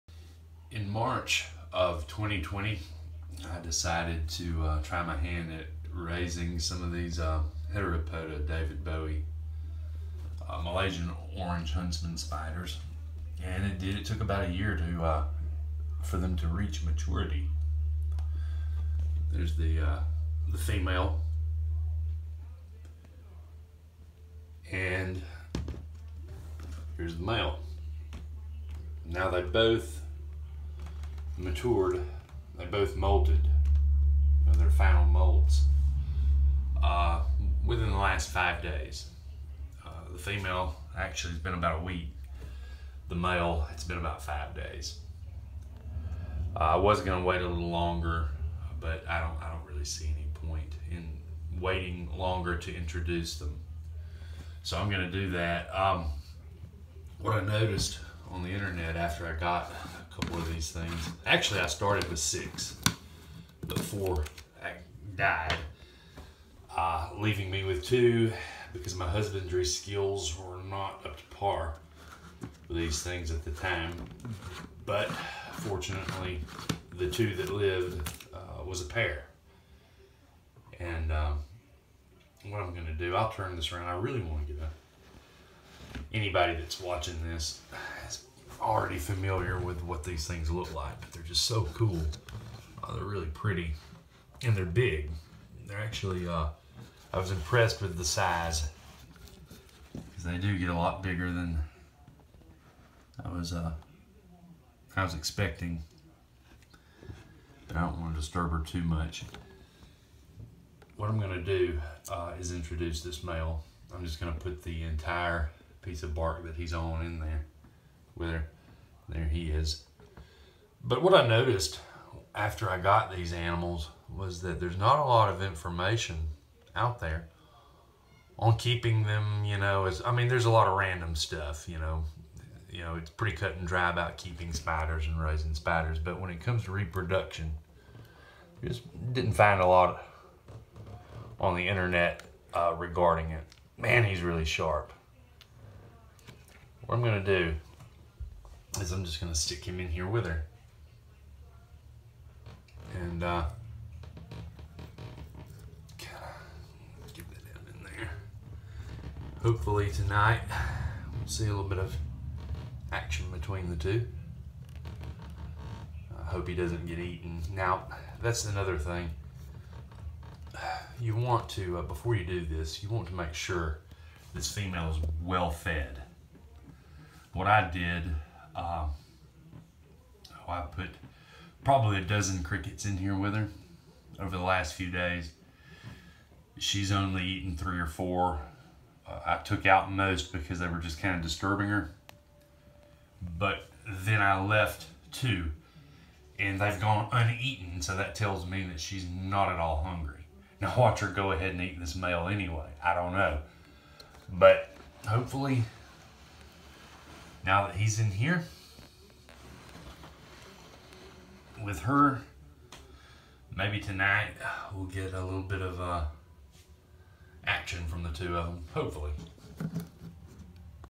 Pairing up my Malaysian orange huntsman spiders Heteropoda davidbowie
Raised this pair of davidbowies from slings. It took nearly a year! Hopefully I’ll get a fertile sac from the female.